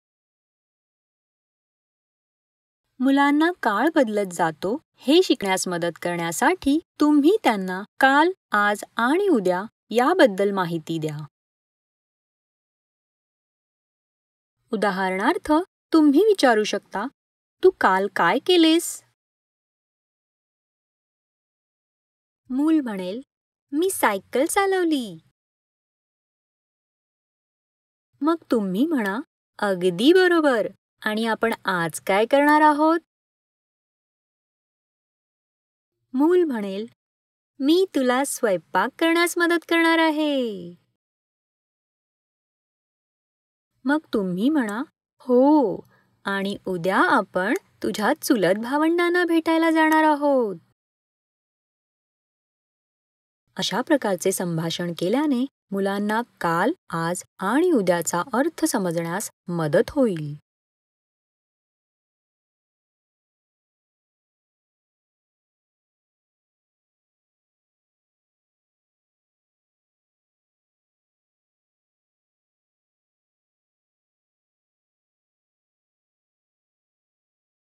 काल, आज, आणि उद्या - Yesterday, Today, and Tomorrow (Marathi)
तुम्ही मुलांना काळ बदलत जातो, आणि काल, आज, आणि उद्या यांचा अर्थ शिकण्यास मदत करू शकता.

Marathi translation by Kalyani Kulkarni and narration by Runali Avinash Patil.

You can help a child learn about the passage of time and the meaning of yesterday, today and tomorrow.